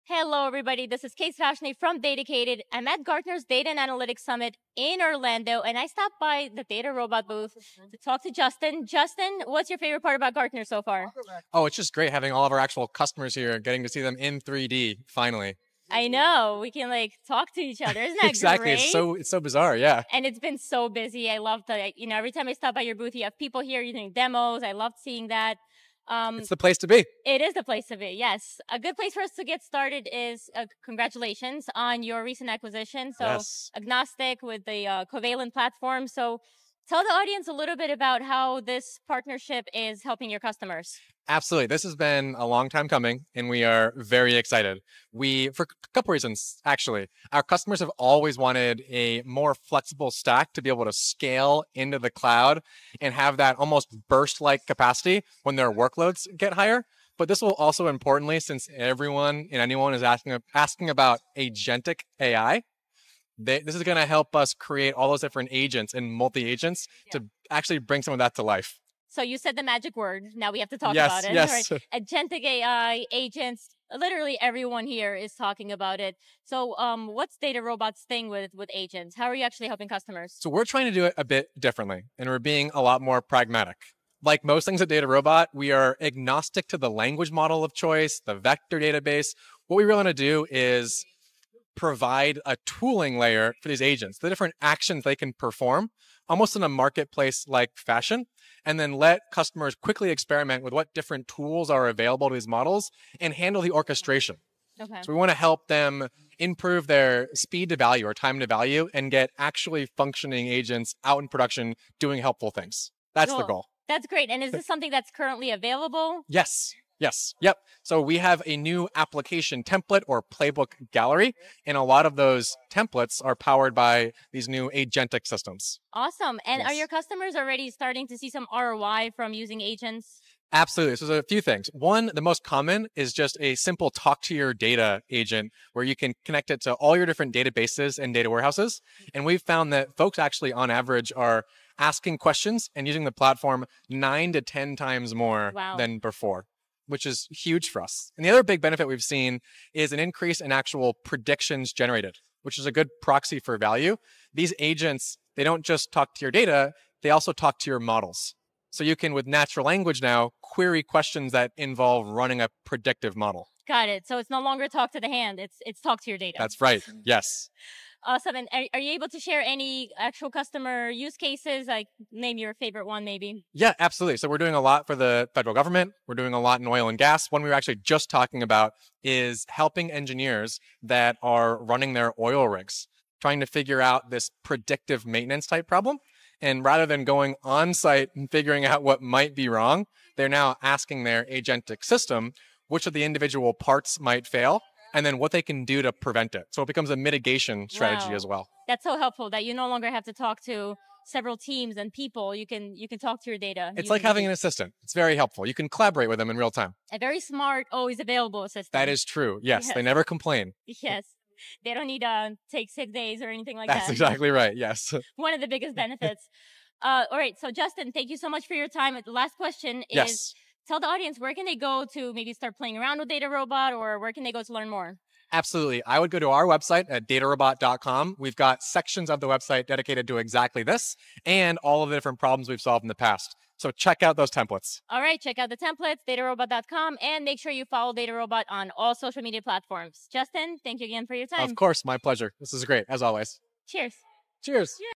How DataRobot is Advancing Agentic AI
Thank you Justin S.(DataRobot) for a great conversation at Gartner's Summit last week. We talked about the recent acquisition of Agnostiq, and how DataRobot is enabling clients get real value from AI agents right now.

Don’t settle for generic AI, check out DataRobot's AI Apps